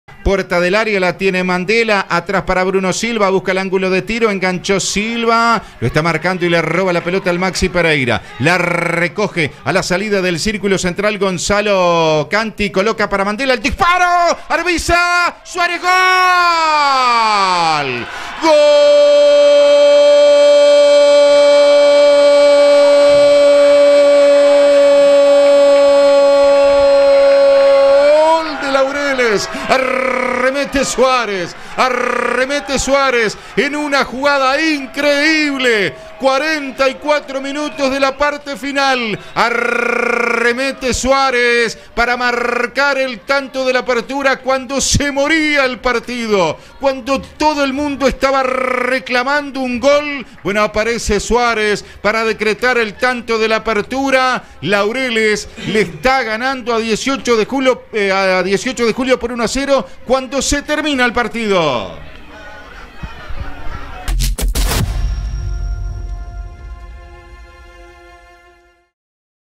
Laureles imparable en la cancha, derrotó a 18 de Julio 1 a 0.
Laureles continua imparable en el torneo de Honor del fútbol fraybentino. Durante casi 70 minutos con 10 jugadores le ganó el clásico a 18 de Julio 1 a 0 con gol sobre el final del partido de Santiago Suarez. Compartimos el gol del cotejo que fue televisado en vivo y directo por RioUruguayTV.